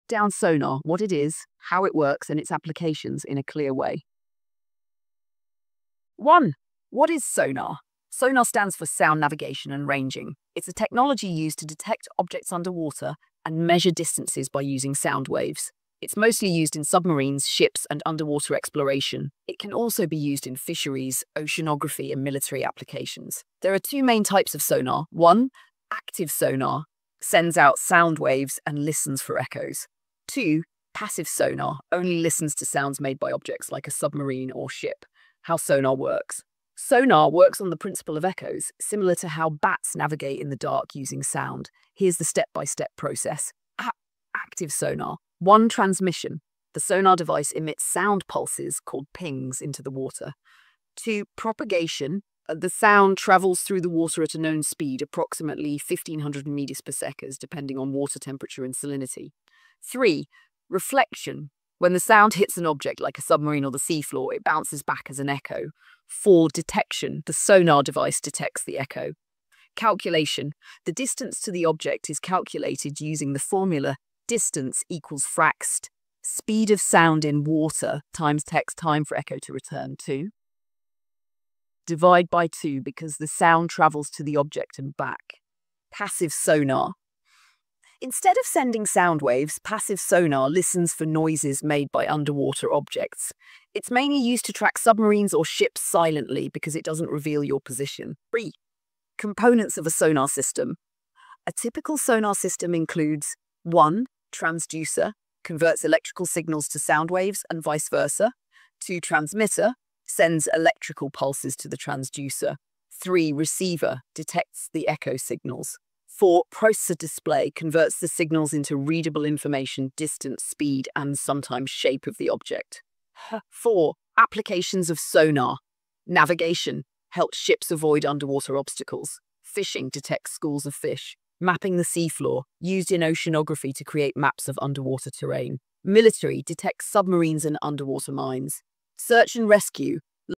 "SONAR: Exploring the Sound Navigation and Ranging Technology"USA
SONAR stands for Sound Navigation and Ranging. It is a technology that uses sound waves to detect, locate, and identify objects underwater. Essentially, SONAR works by emitting sound pulses into the water and then listening for echoes that bounce back from objects. By measuring the time it takes for the echo to return, SONAR systems can determine the distance, size, and shape of objects